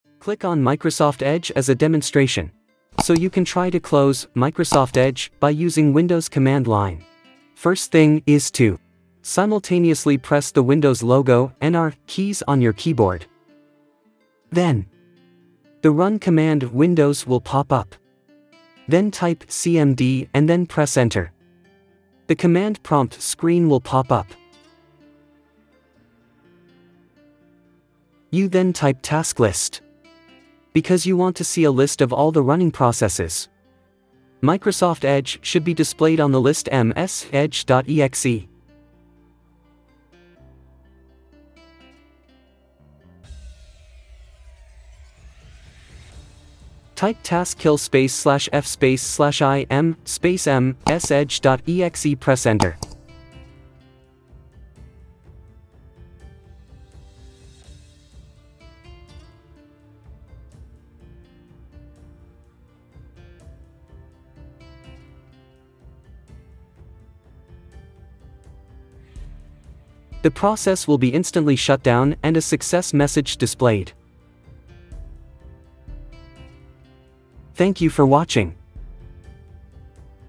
How to Kill the Microsoft Edge With the Windows Command Line.
Tasklist tells all the running processes
Taskkill   /f   /im msedge.exe   "is used to terminate the Microsoft Edge"
The /f switch forces taskkill to close the program
The /im msedge.exe is a part of the command to recognise the program to close.